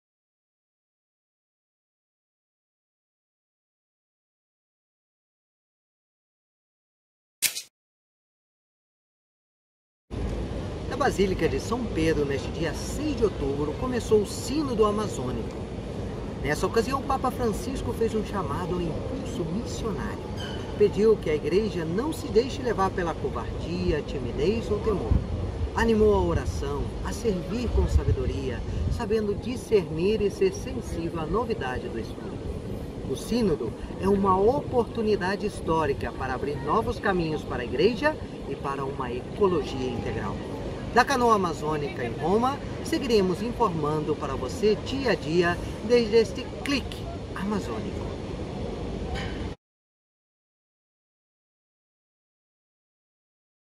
SinodoAmazonico - ClickAmazonico No.1 - Portugués
Começou o SinodoAmazonico: é uma oportunidade histórica para abrir novos caminhos para a Igreja e para uma ecologia integral.